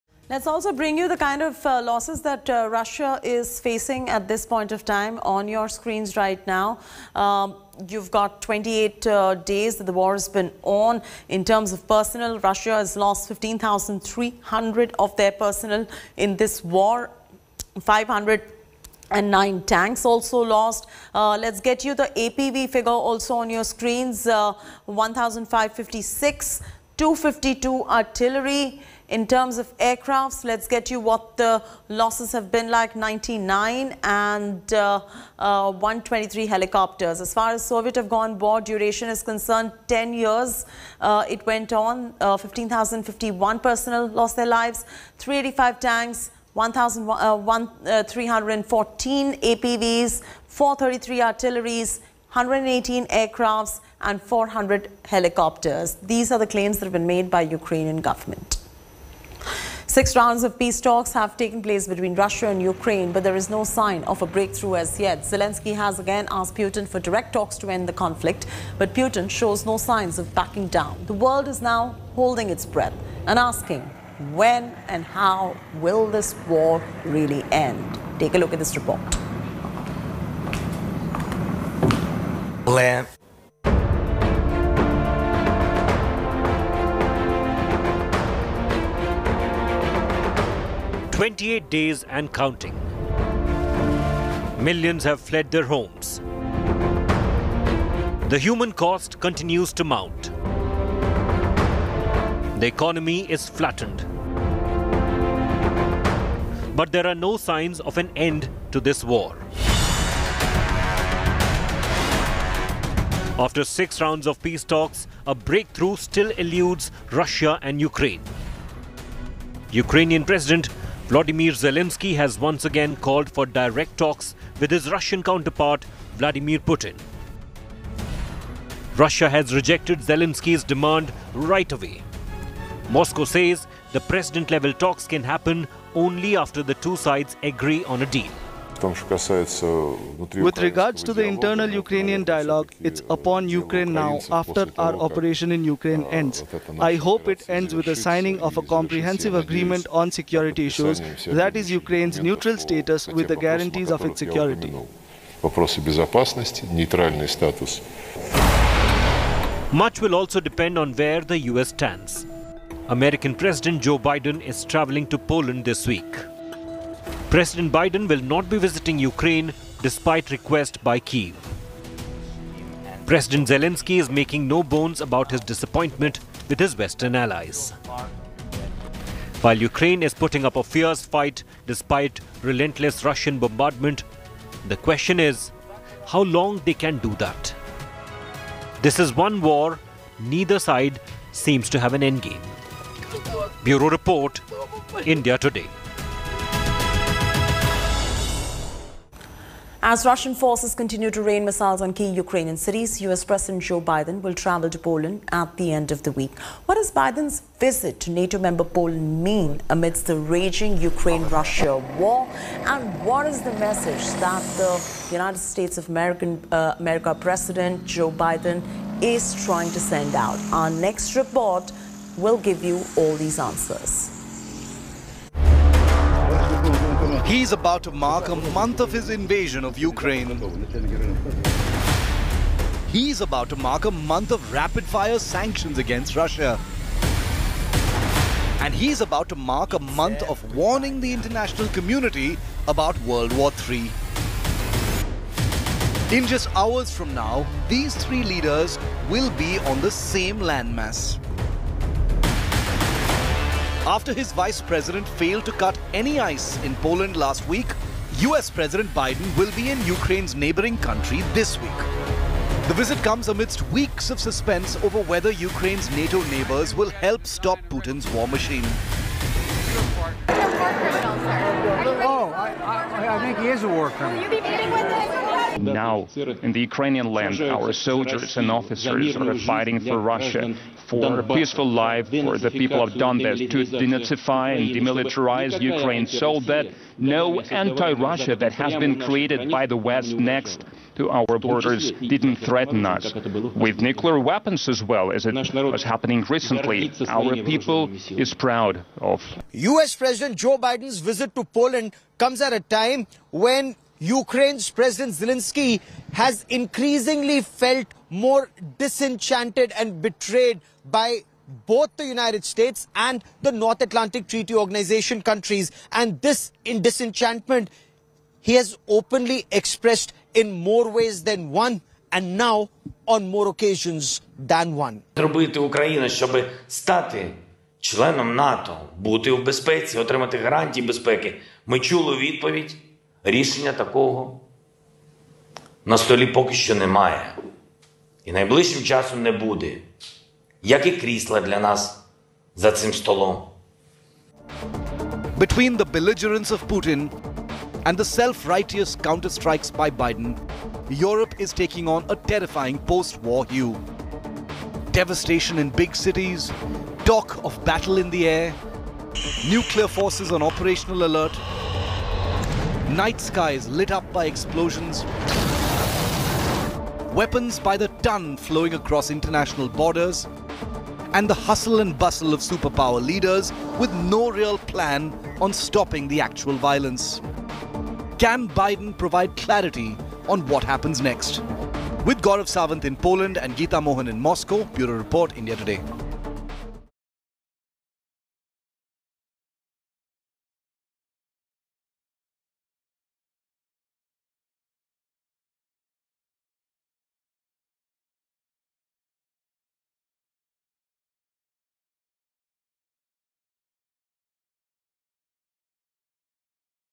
Loss Of Mighty Russia Amid This Raging War With Ukraine Take A Look | Russia-Ukraine Stand-off
As Russia is continuously spewing its wrath on Ukraine, here we have brought you all the details of the loss that mighty Russia has faced throughout the waging war with Ukraine. Have a look.